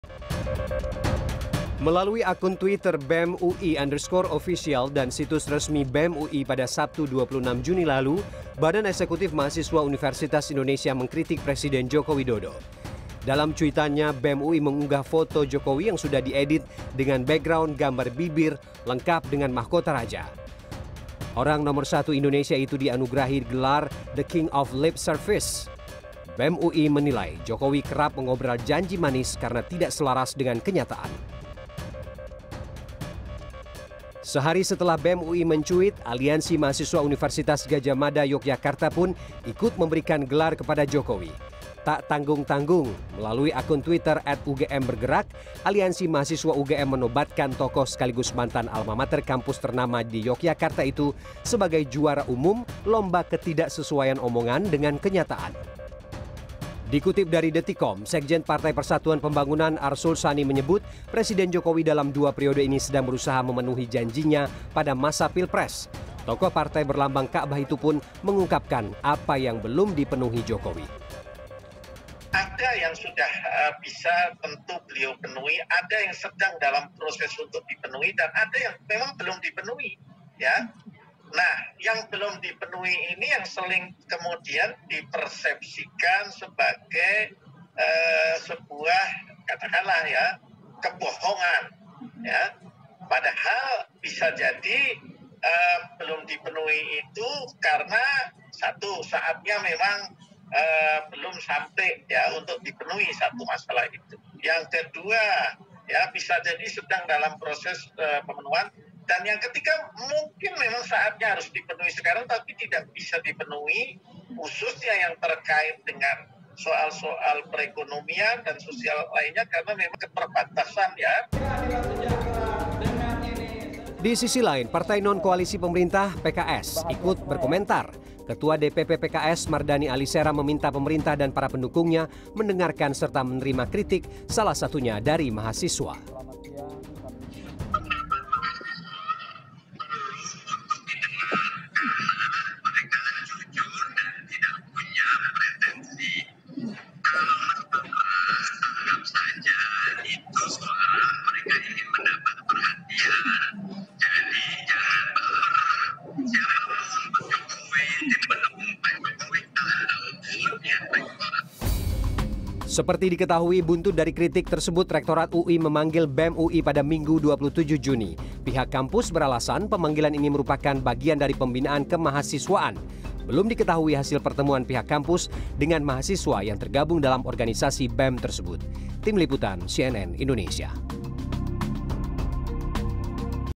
Kritik Jokowi, Cuitan Mahasiswa Tuai Polemik
Badan eksekutif mahasiswa universitas Indonesia mengkritik presiden Joko Widodo, lantaran sering mengobral janji manis, yang kerap tidak direalisasi. Jokowi dicap sebagai The King Of Lip Service, alias raja membual.

Partai koalisi pendukung presiden memastikan, Jokowi akan tetap menepati janjinya, meski membutuhkan proses.